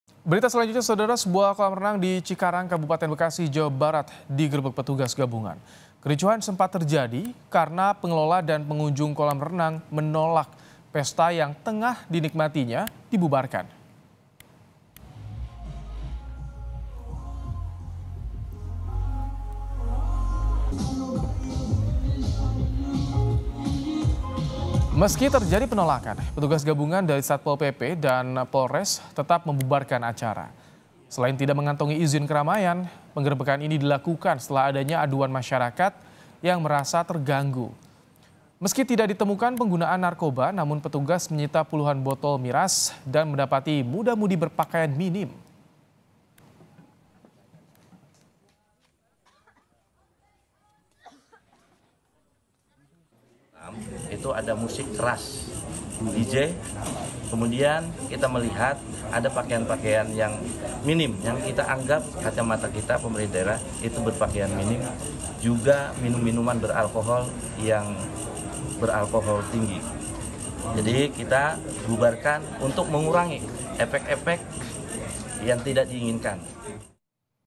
Tak Kantongi Izin dan Ganggu Warga, Kolam Renang yang Adakan Pesta Miras Digerebek
BEKASI, KOMPAS.TV - Sebuah kolam renang di Cikarang, Kabupaten Bekasi, Jawa Barat, digerebek petugas gabungan.

Kericuhan sempat terjadi, karena pengelola dan pengunjung kolam renang menolak pesta yang tengah dinikmatinya dibubarkan.

Meski terjadi penolakan, petugas gabungan dari Satpol PP dan Polres tetap membubarkan acara.

Selain tidak mengantongi izin keramaian, penggerebekan ini dilakukan setelah adanya aduan masyarakat yang merasa terganggu.

Meski tidak ditemukan penggunaan narkoba namun petugas menyita puluhan botol miras dan mendapati muda-mudi berpakaian minim.